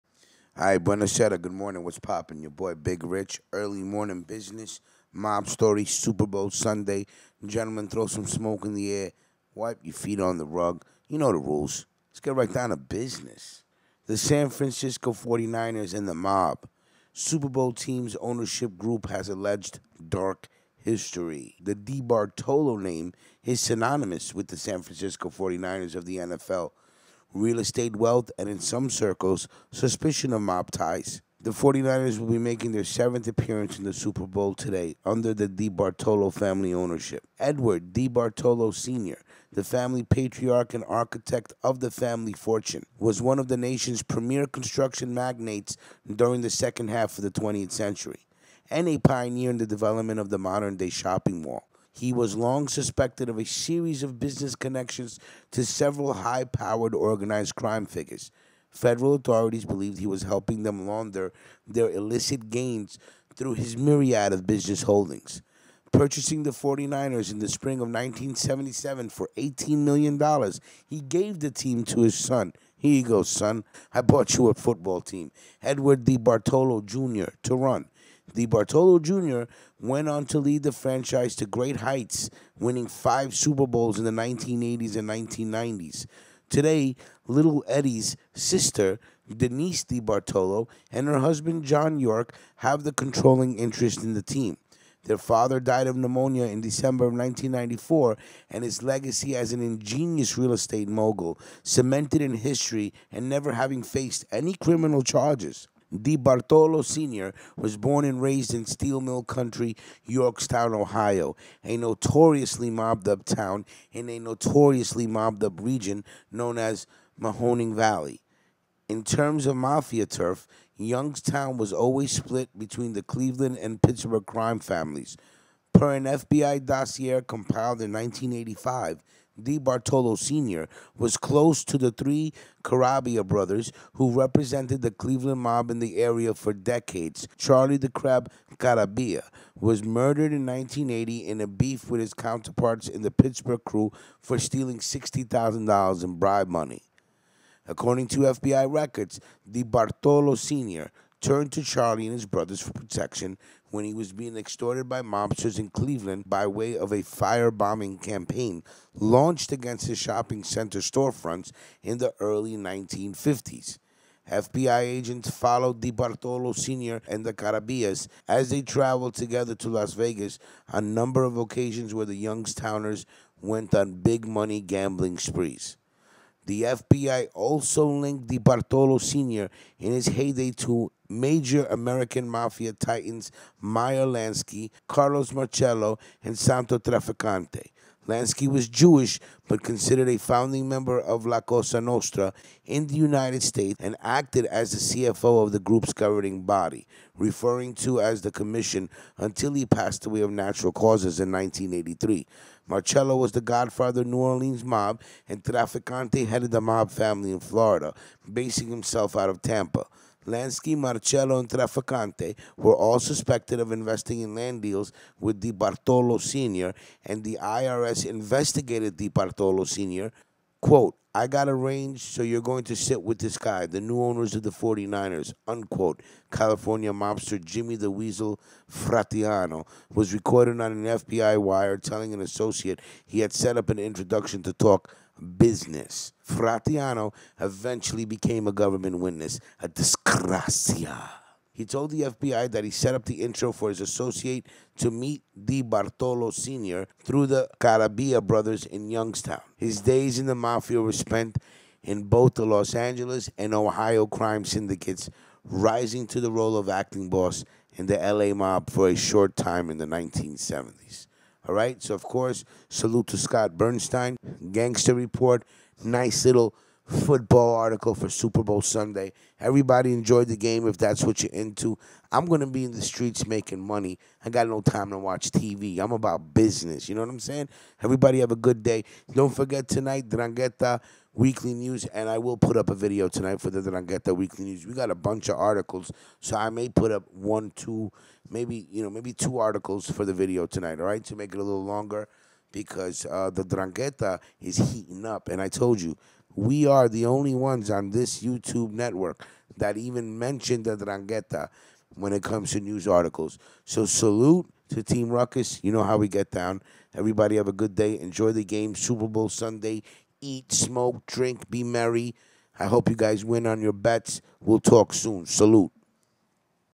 THE SAN FRANCISCO 49ERS & THE MOB SUPER BOWL TEAM’S OWNERSHIP GROUP HAS ALLEGED DARK HISTORY
Edward DeBartolo, Sr., the family patriarch and architect of the family fortune, was one of the nation’s premier construction magnates during the second half of the 20th Century and a pioneer in the development of the modern day shopping mall. He was long suspected of a series of business connections to several high-powered organized crime figures – federal authorities believed he was helping them launder their illicit gains through his myriad of business holdings